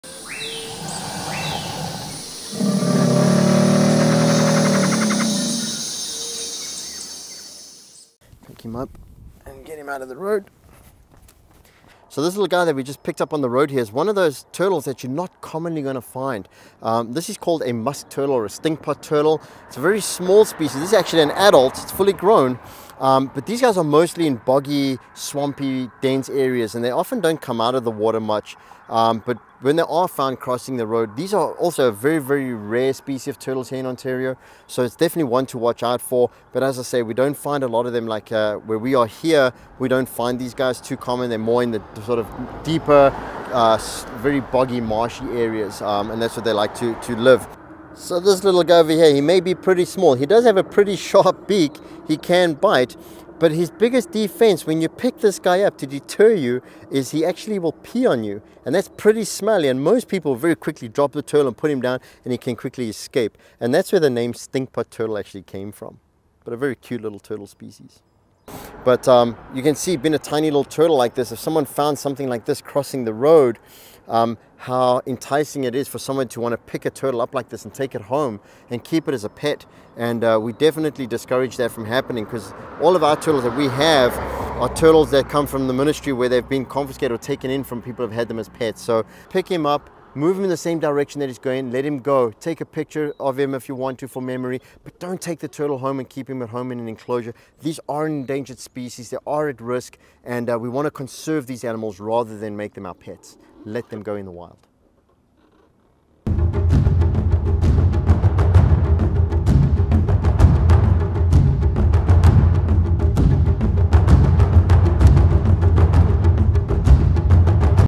The Stink Pot Turtle?
Lee Parker tells us a little something about a little turtle - the stink pot turtle! Please! Always be safe stopping to help turtles cross the road.